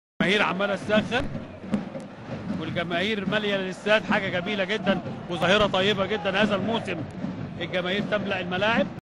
Khaled & Zeyad @ Mokaweloon Stadium (Zamalek vs Mokaweloon)
Zamalek vs Mokaweloon 2-2 (Egyptian League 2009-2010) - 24 August 2009 - 3rd Week